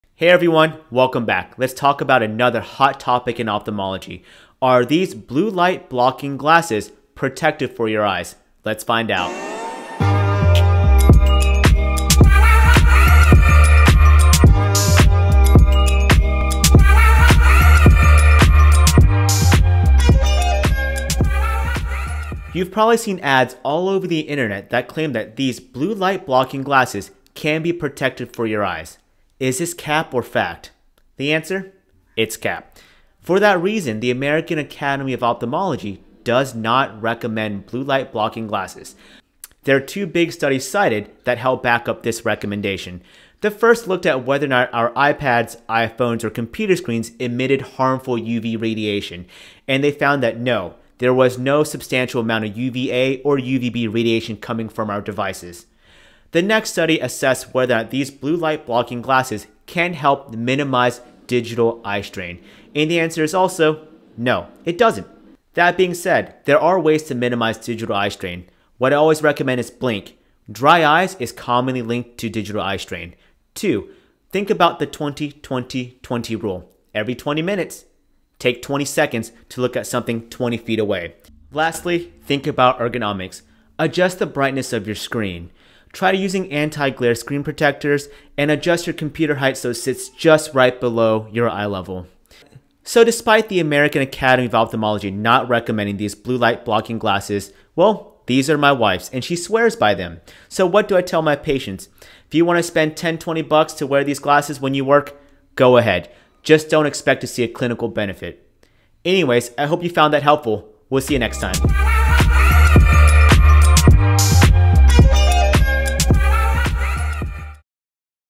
Are Blue Light Blocking Glasses Worth It? Hint - Nope // Your Eyes Tell
You may have heard the hype about blue blocking glasses. Do they actually make a difference? Absolutely not. The American Academy of Ophthalmology does NOT recommend it.